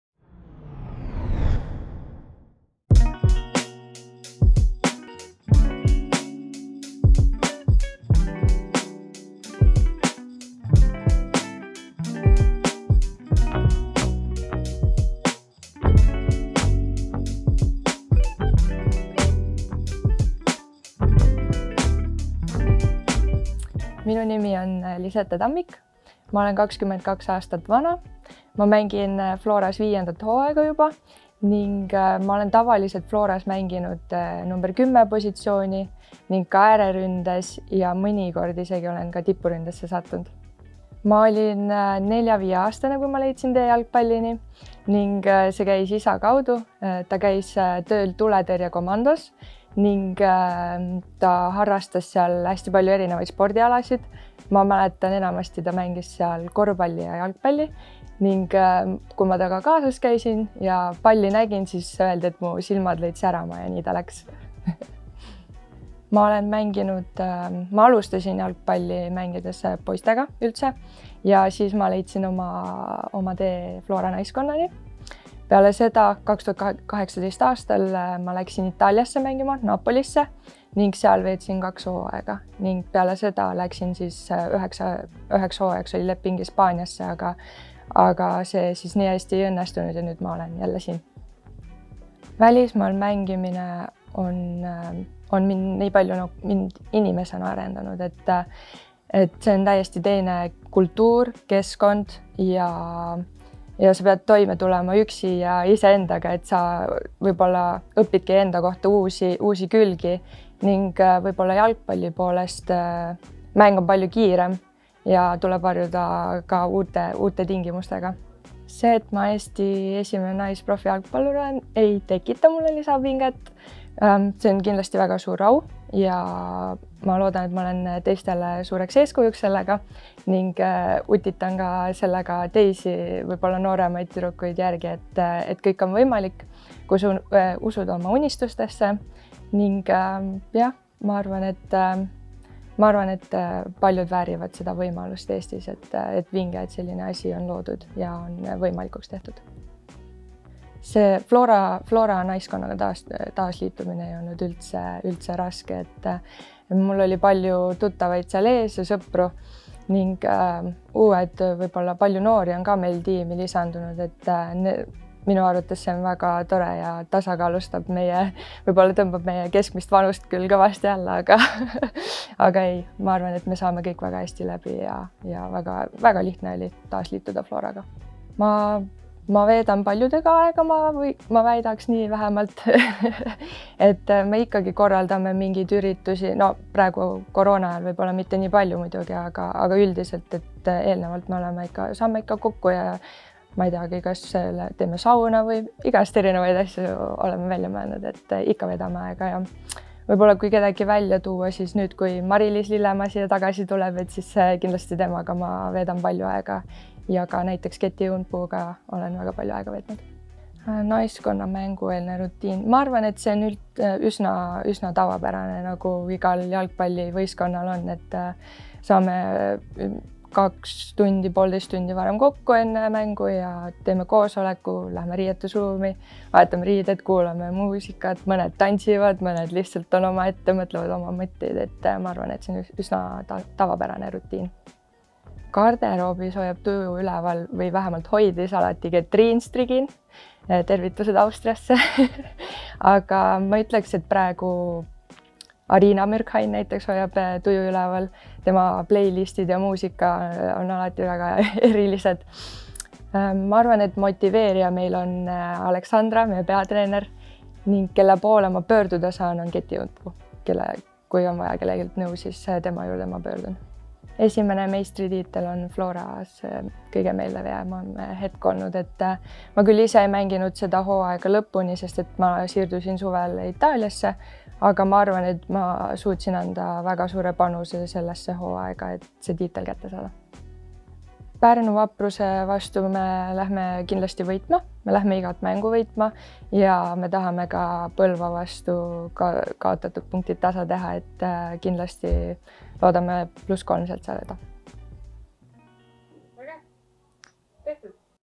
Mängupäeva staari tutvustus: Lisette Tammik
Sissejuhatus FC Flora vs Pärnu JK Vapruse mängule, kus Lisette Tammik räägib, mida talle välismaal mängimine on õpetanud, tänu kellele ta üldse jalgpalli juurde jõudis ning milliste emotsioonidega nad kohtumisele vastu lähevad!💚